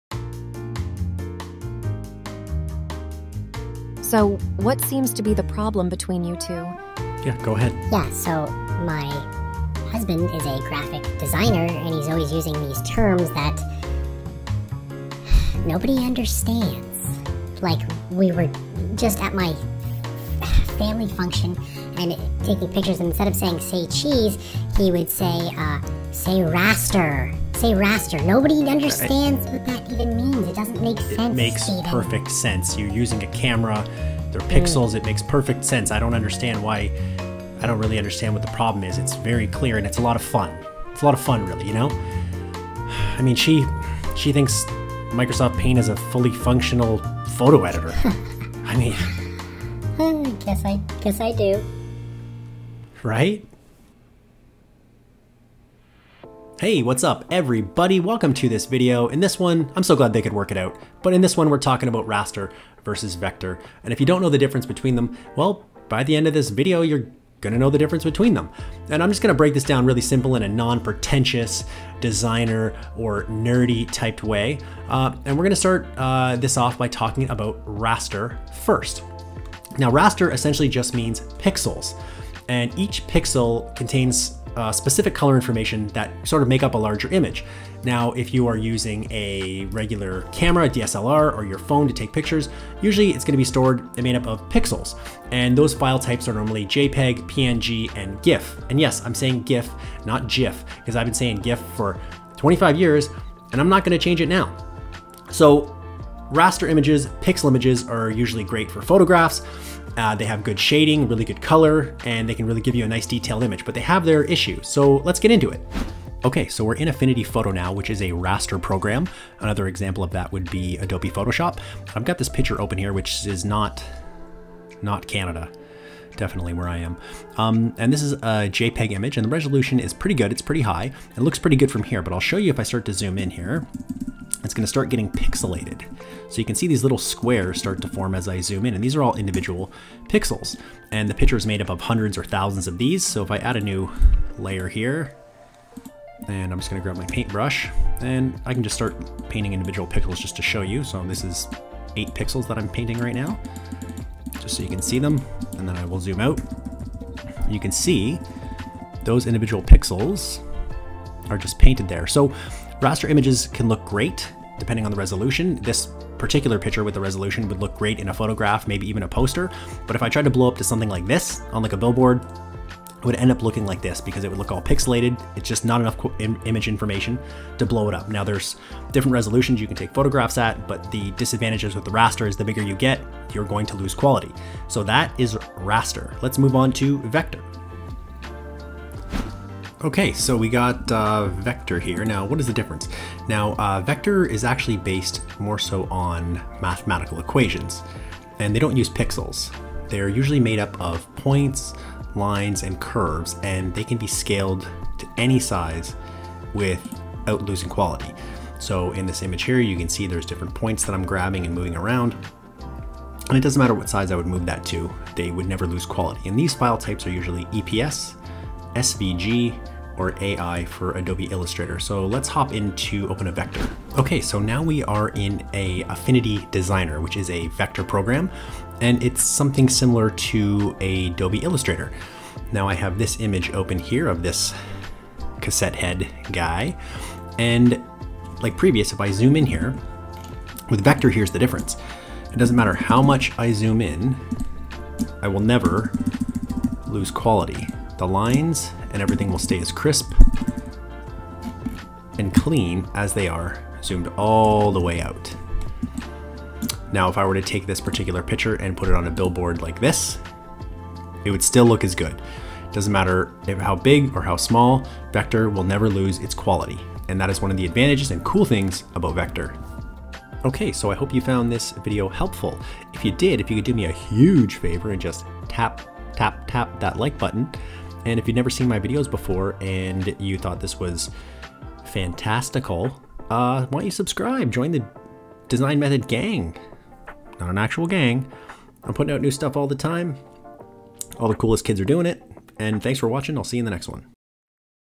Vector Vs. Raster: What's the Difference?
In this one we're discussing the difference between Vector graphics and Raster graphics.

Let's go!